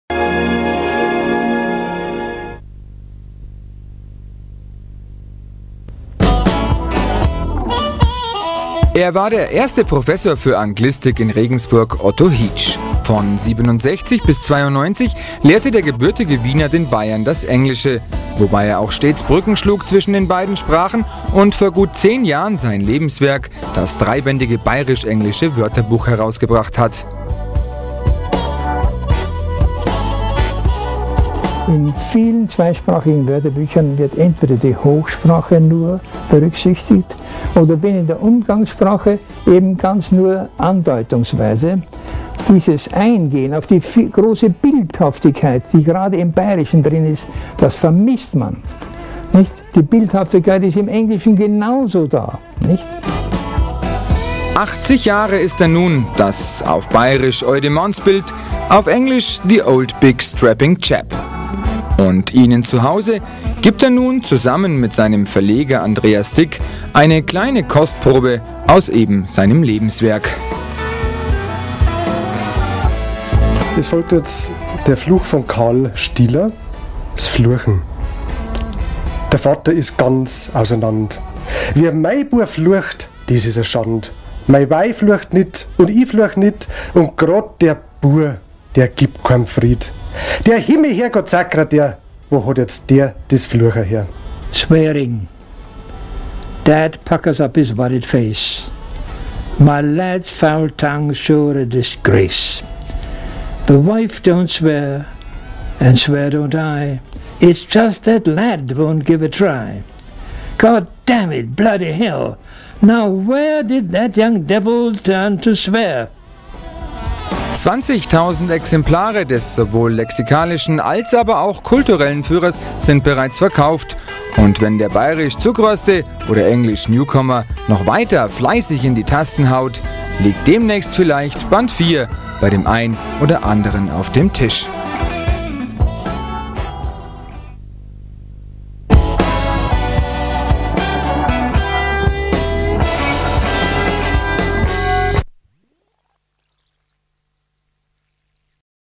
Portrait Prof. Hietsch.avi
Kurzportrait über den ersten Anglistik Professor der Universität Regensburg anlässlich seiner Veröffentlichung seines Bayerisch - Englischen Wörterbuches